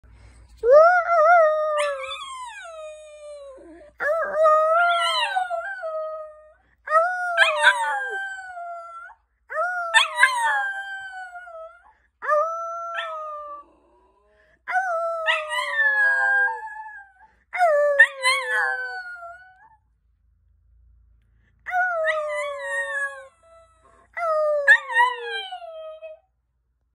How to make your Dog howl
So here I finally was able to get Ozzy to howl by howling 😂